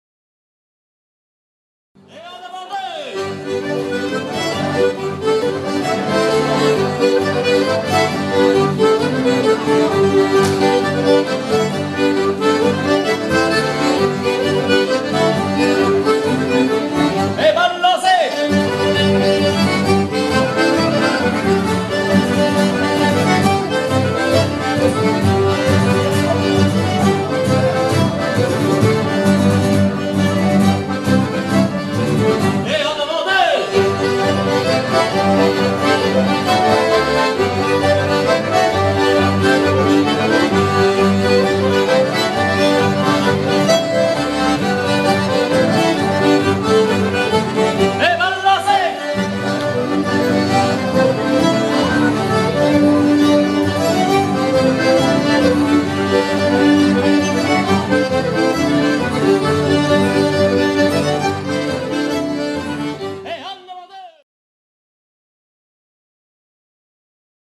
Per'Cordanche - Bal à Béruges - Suite d'avant-deux
Bal à Béruges (86), salle des fêtes, le samedi 15 mars, organisé par la section "Danses Populaires" de la FEPS. Ici, une suite d'avant-deux, au tout début du bal, par les musiciens du groupe.